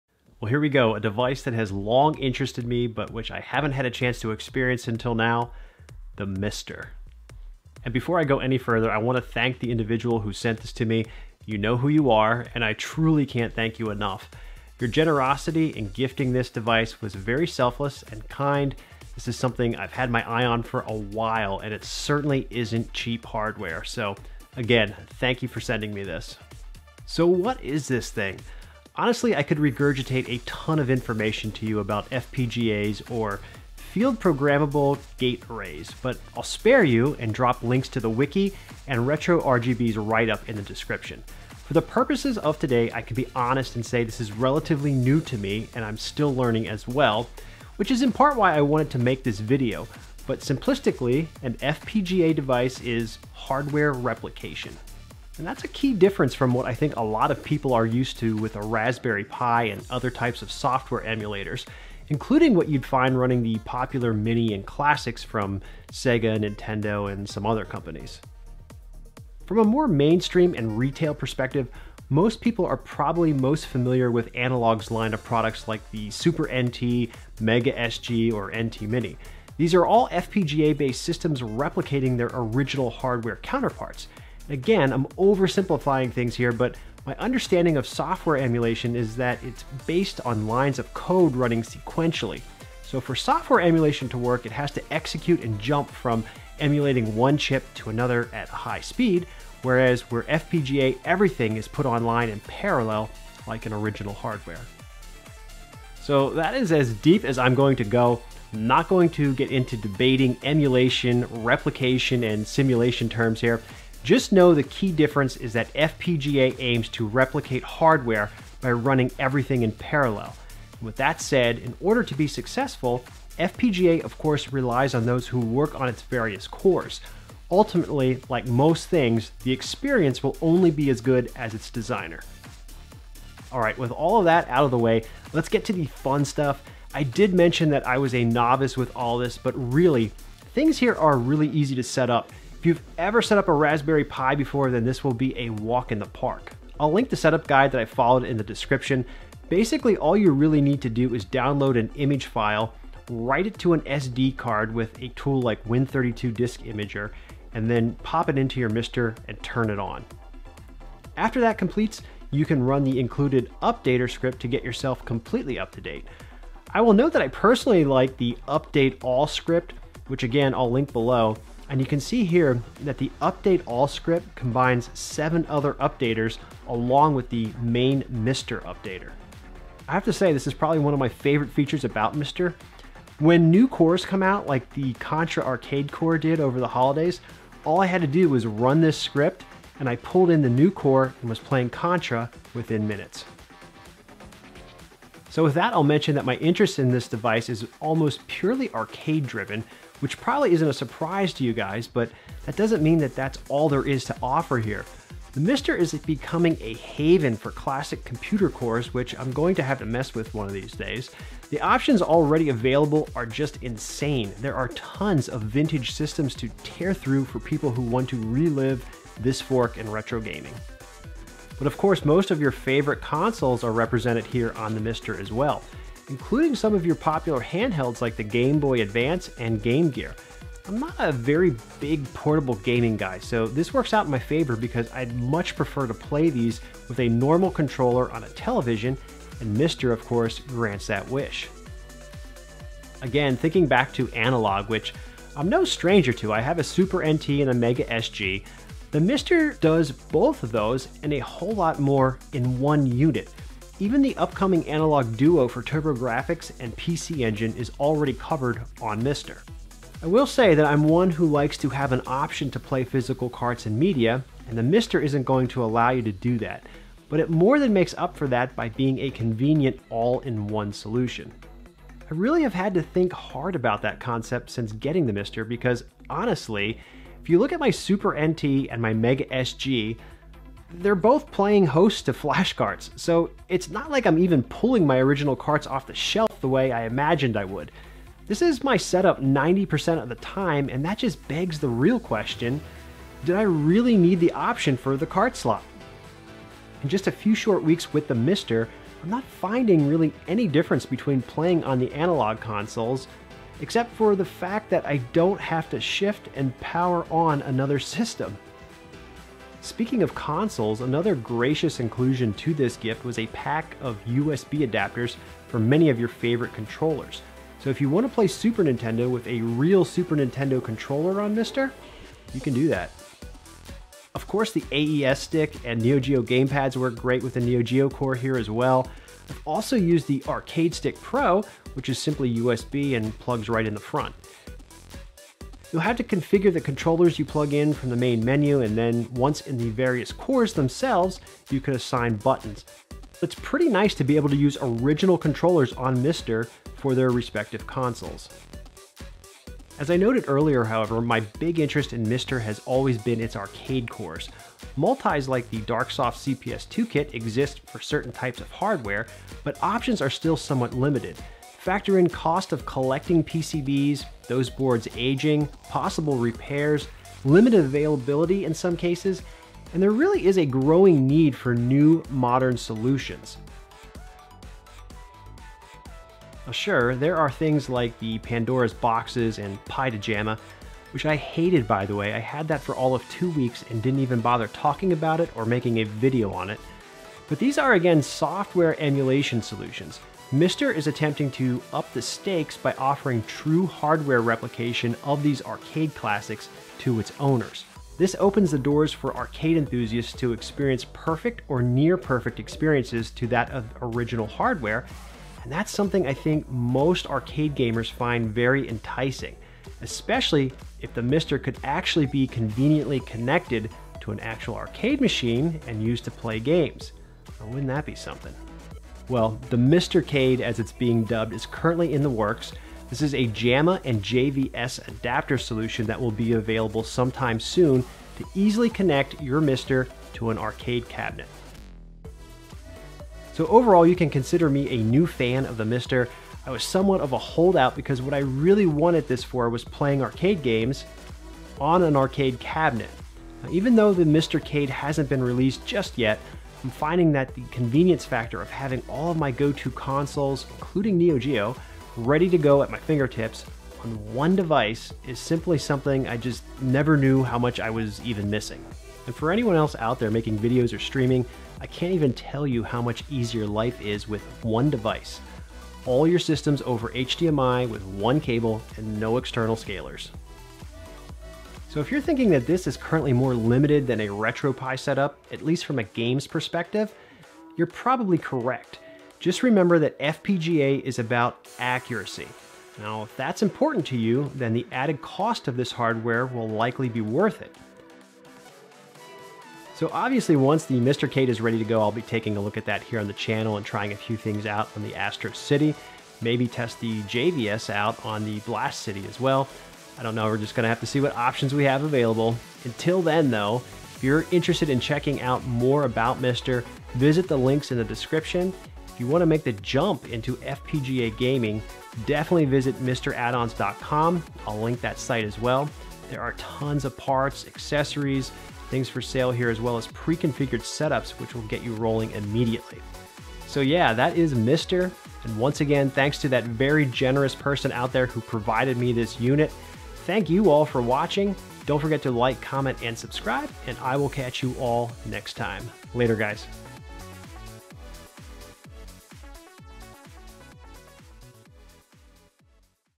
MiSTer FPGA Review 2021: Arcade & Console Accuracy!
I suppose it's better late than never.  Thanks to the generosity of a gracious individual I've been able to take a look the growing ever more popular MiSTer FPGA hardware.  It's goal of arcade accuracy has long been an interest of mine but I've been unable to experience this first hand until now.  Oh, and there is a little thing called the MiSTerCade on it's way as well.